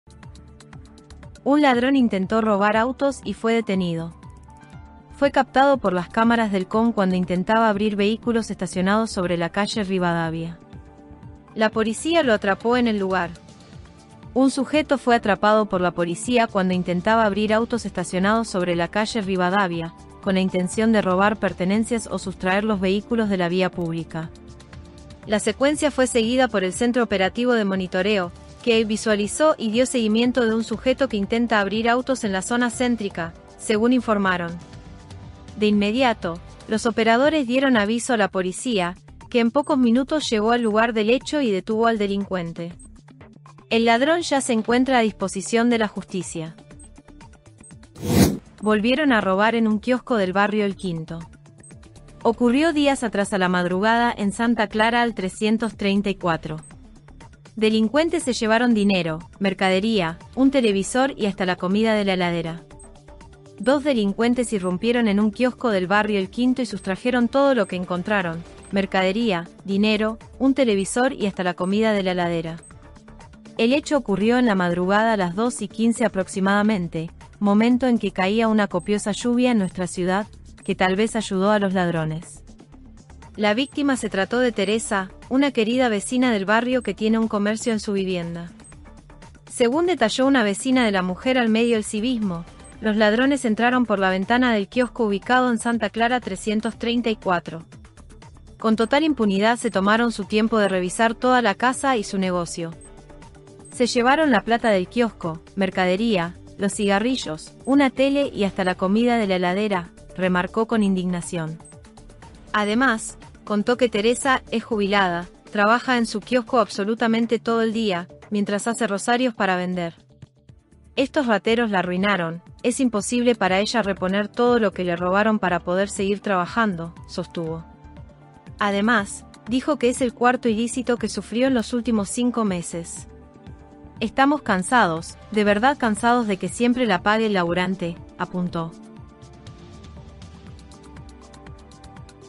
LUJAN | Robos e intento de robos en diferentes zonas de lujan: EL VIDEO
Un ladrón intentó robar autos y fue detenido
Fue captado por las cámaras del COM cuando intentaba abrir vehículos estacionados sobre la calle Rivadavia. La Policía lo atrapó en el lugar.
Un sujeto fue atrapado por la Policía cuando intentaba abrir autos estacionados sobre la calle Rivadavia, con la intención de robar pertenencias o sustraer los vehículos de la vía pública.
La secuencia fue seguida por el Centro Operativo de Monitoreo, que “visualizó y dio seguimiento de un sujeto que intenta abrir autos en la zona céntrica”, según informaron.
De inmediato, los operadores dieron aviso a la Policía “que en pocos minutos llegó al lugar del hecho y detuvo al delincuente. El ladrón ya se encuentra a disposición de la justicia.

Volvieron a robar en un kiosco del barrio El Quinto
Ocurrió dias atras a la madrugada en Santa Clara al 334. Delincuentes se llevaron dinero, mercadería, un televisor y hasta la comida de la heladera.
Dos delincuentes irrumpieron en un kiosco del barrio El Quinto y sustrajeron todo lo que encontraron: mercadería, dinero, un televisor y hasta la comida de la heladera.
El hecho ocurrio en la madrugada a las 2.15 aproximadamente, momento en que caía una copiosa lluvia en nuestra ciudad, que tal vez ayudó a los ladrones. La víctima se trató de Teresa, una querida vecina del barrio que tiene un comercio en su vivienda.
Según detalló una vecina de la mujer al medio EL CIVISMO, los ladrones entraron por la ventana del kiosco ubicado en Santa Clara 334. “Con total impunidad se tomaron su tiempo de revisar toda la casa y su negocio. Se llevaron la plata del kiosco, mercadería, los cigarrillos, una Tele y hasta la comida de la heladera”, remarcó con indignación. Además, contó que Teresa “es jubilada, trabaja en su kiosco absolutamente todo el día, mientras hace rosarios para vender”.
“Estos rateros la arruinaron, es imposible para ella reponer todo lo que le robaron para poder seguir trabajando”, sostuvo. Además, dijo que es el cuarto ilícito que sufrió en los últimos cinco meses. “Estamos cansados, de verdad cansados de que siempre la pague el laburante”, apuntó.